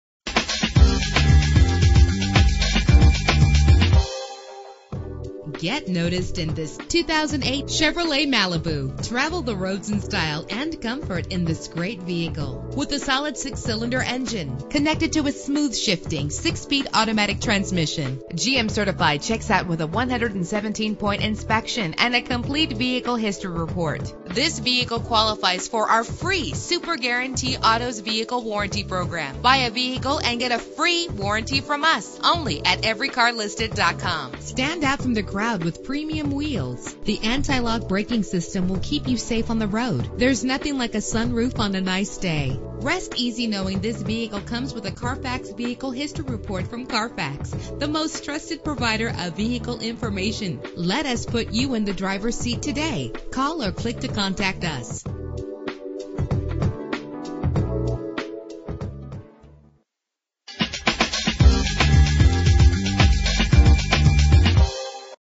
2008 Chevrolet Malibu - Muskegon MI
Year: 2008
Make: Chevrolet
Model: Malibu
Trim: LTZ
Engine: 3.6 liter 6 cylinder 24 valve
Transmission: 6-Speed Automatic
Color: Red
Mileage: 44738
Address:
2474 Henry St.